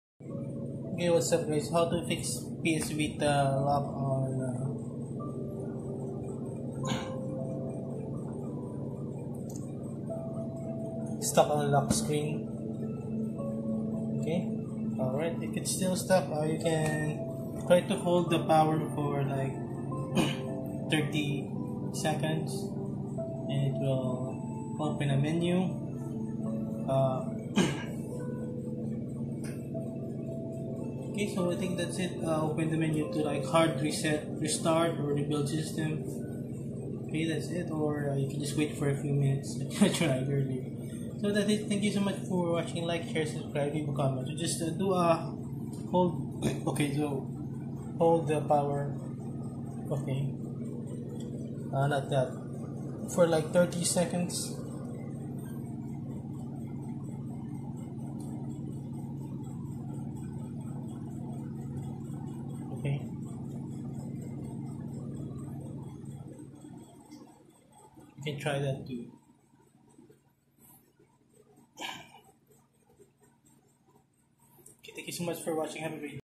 HOW TO FIX PSVITA STUCK ON LOCK SCREEN
Screen lock issue solved easy Playstation vita portable console stuck on start screen saver